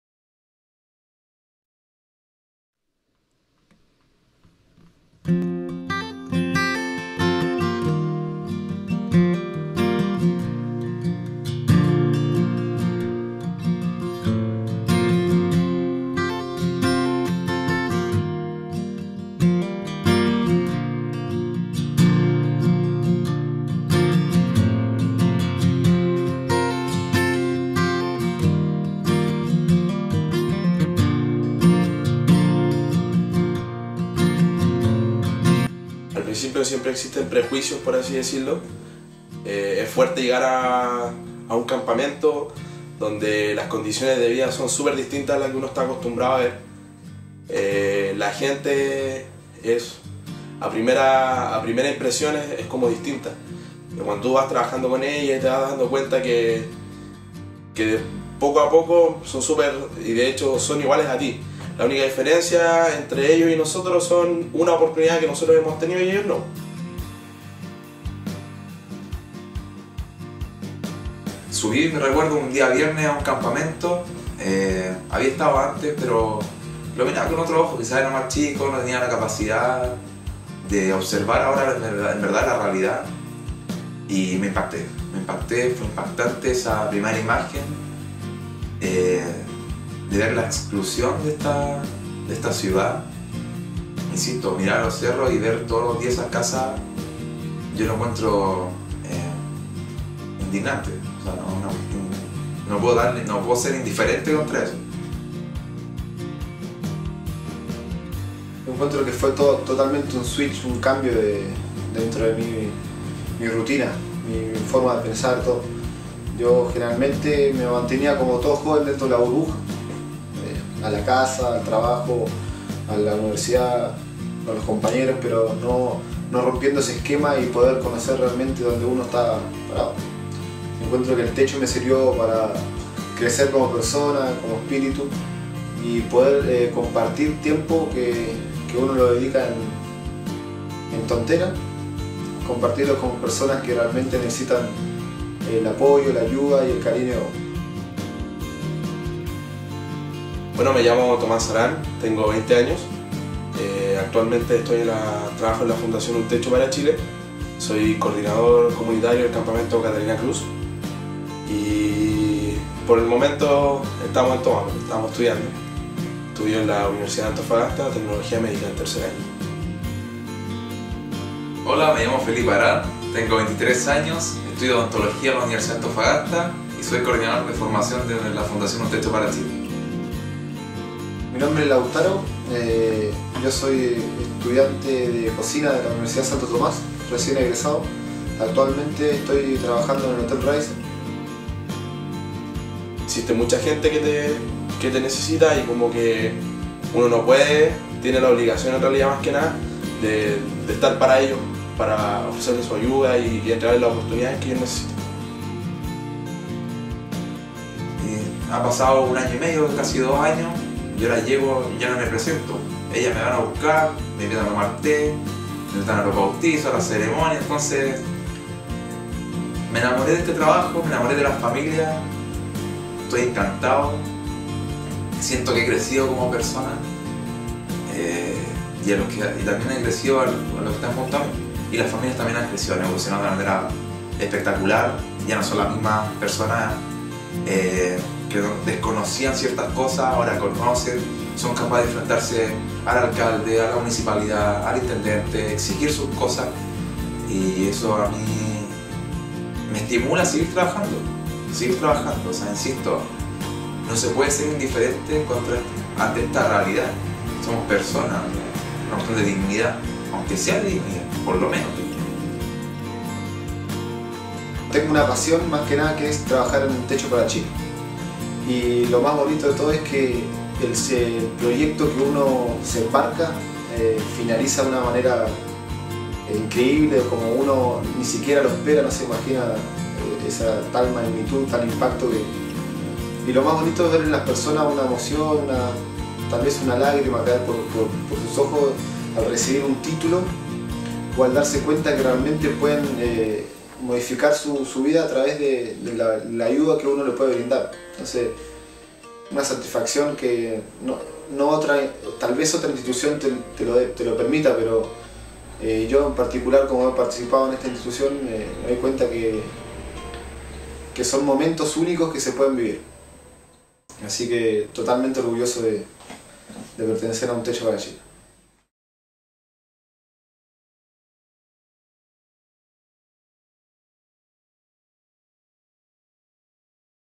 Un Techo Para Chile (Antofagasta)
Emotivo Testimonio de Jóvenes Voluntarios de UTPCH Antofagasta. Escucha como cambiaron sus vidas al pertenecer a esta fundación.

En Todo Amar y Servir. Yo Voluntario.
Antofagasta 2011.